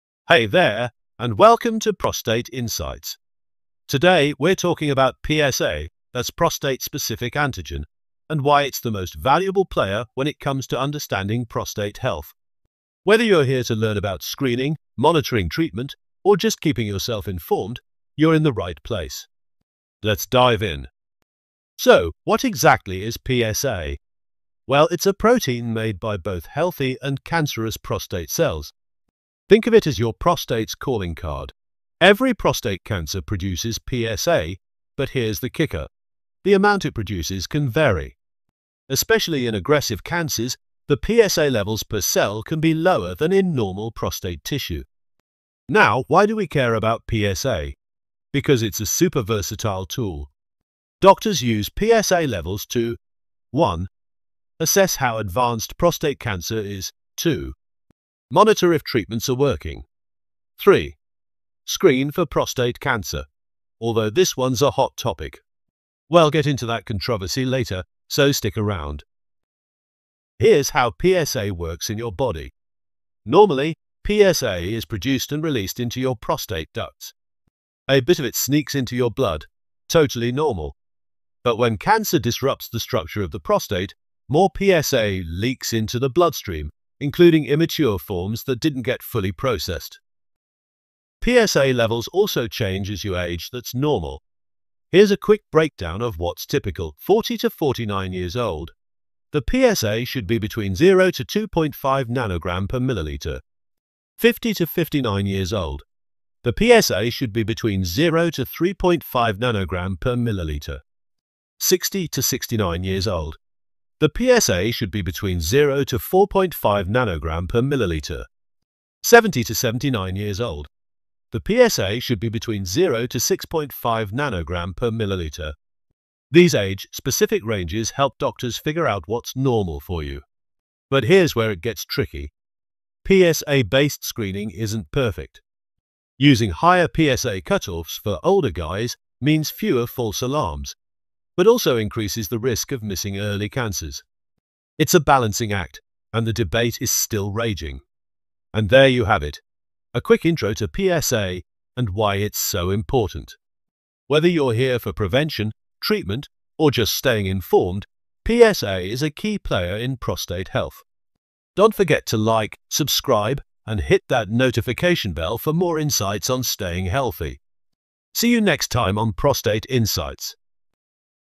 PSA Values by Age EXPLAINED for Optimal Health!
In this video, we'll explain PSA values by age to help you understand what is considered normal and what may indicate potential health concerns like prostate cancer. Learn more about the importance of PSA tests for optimal health!